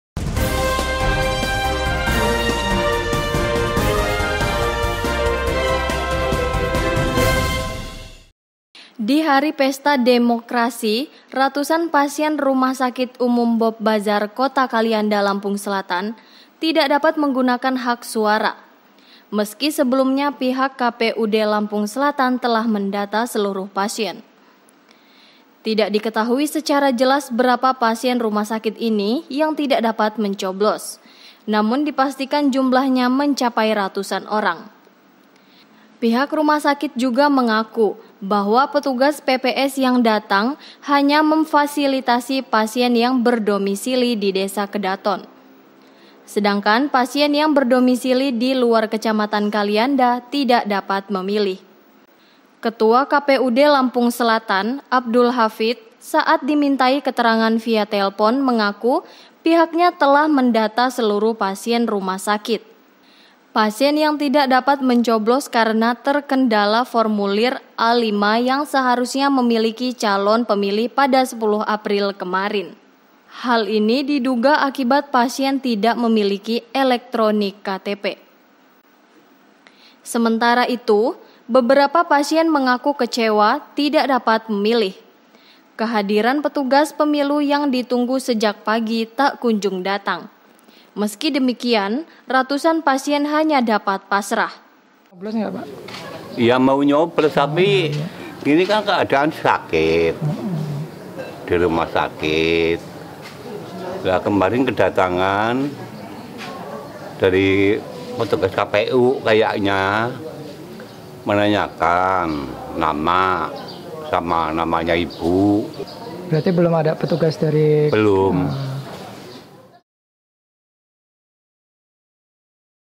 Ratusan Pasien Rumah Sakit Kehilangan Hak Suara
Saburai TV - Lampung